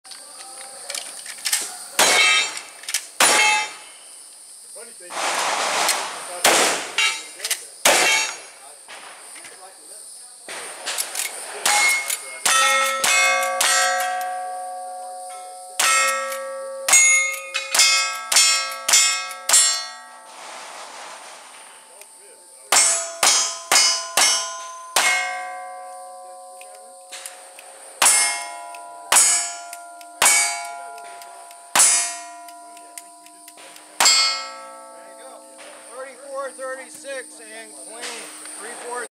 Stage 2 Madame Moog LCP 7 20 19
The Lake County Pistoleros hosted their July Monthly Match at Eustis Gun Club in Tavares, FL on July 20, 2019.  Santa Fe River Stan was the Overall Top Gun and Madame Moog the Lady's Top Gun with Delta Glen, Dang It Dan, Cypress Sun, and Amaduelist rounding out the Top 5.